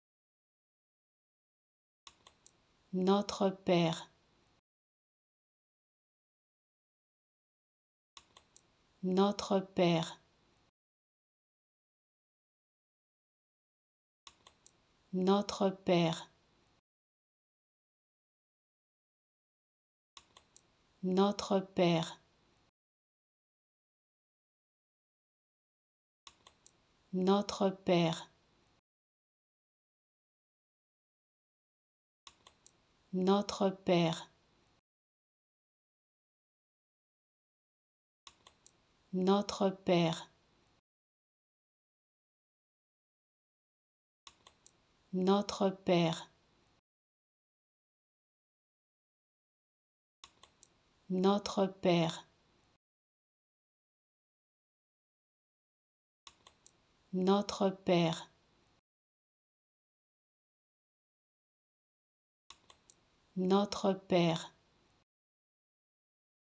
How to Pronounce ''Notre Père'' (Our Father) Correctly in French
Learn how to say and properly pronounce ''Notre Père'' in French with this free pronunciation tutorial. Apprenez à prononcer des mots en français grâce à nos tutoriels.